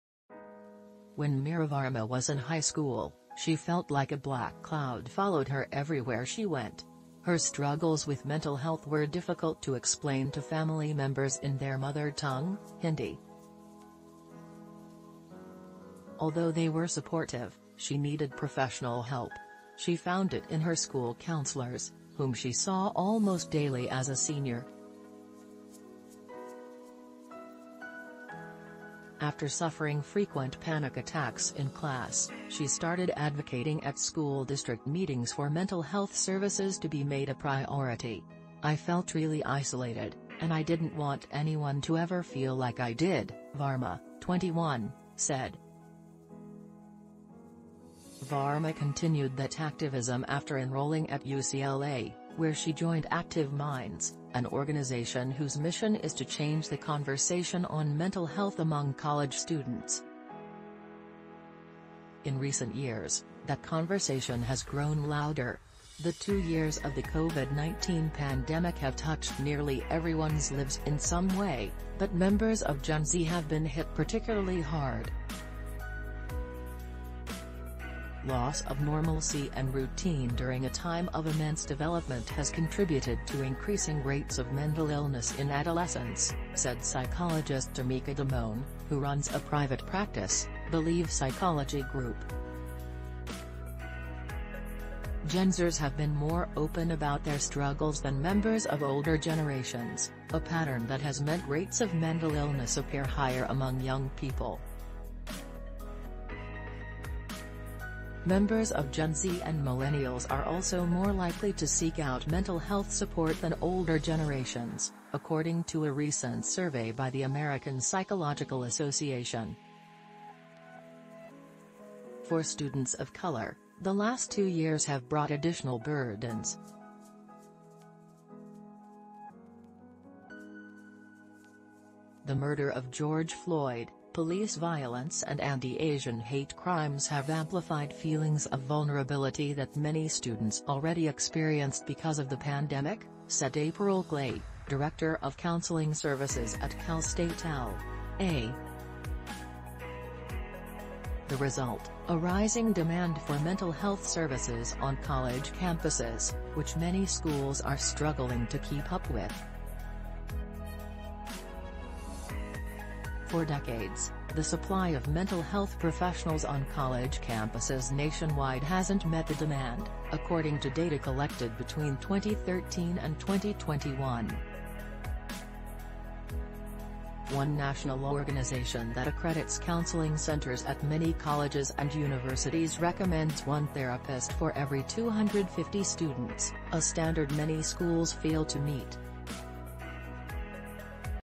Gen Z students want better mental health care access on campus
When Meera Varma was in high school, she felt like a black cloud followed her everywhere she went. Her struggles with mental health were difficult to explain to family members in their mother tongue, Hindi. Although they were supportive, she needed professional help. She found it in her school counselors, whom she saw almost daily as a senior. After suffering frequent panic attacks in class, she started advocating at school district meetings for mental health services to be made a priority.“I felt really isolated, and I didn’t want anyone to ever feel like I did, ” Varma, 21, said. Varma continued that activism after enrolling at UCLA, where she joined Active Minds, an organization whose mission is to change the conversation on mental health among college students. In recent years, that conversation has grown louder. The two years of the COVID-19 pandemic have touched nearly everyone’s lives in some way, but members of Gen Z have been hit particularly hard. Loss of normalcy and routine during a time of immense development has contributed to increasing rates of mental illness in adolescents, said psychologist Tamika Damond, who runs a private practice, Believe Psychology Group. Gen Zers have been more open about their struggles than members of older generations, a pattern that has meant rates of mental illness appear higher among young people. Members of Gen Z and millennials are also more likely to seek out mental health support than older generations, according to a recent survey by the American Psychological Assn. For students of color, the last two years have brought additional burdens. The murder of George Floyd, police violence and anti-Asian hate crimes have amplified feelings of vulnerability that many students already experienced because of the pandemic, said April Clay, director of counseling services at Cal State L. A. The result: a rising demand for mental health services on college campuses, which many schools are struggling to keep up with. For decades, the supply of mental health professionals on college campuses nationwide hasn’t met the demand, according to data collected between 2013 and 2021. One national organization that accredits counseling centers at many colleges and universities recommends one therapist for every 250 students, a standard many schools fail to meet. Campuses with fewer counselors might end up with long waiting lists and offer only a limited number of sessions to students in an effort to spread resources among as many as possible.“The mental health crisis has actually been happening for a number of years, building up to all of the racial trauma and the pandemic, ” said Dr. Frances Diaz, director of the Counselor Center at UC Irvine. “And what we we’re seeing across the country is — university counseling centers in particular — now have demands that exceed the capacity to meet those demands. ”Many schools would need to nearly double their staff to meet the ratio the accrediting group recommends. Cal State L.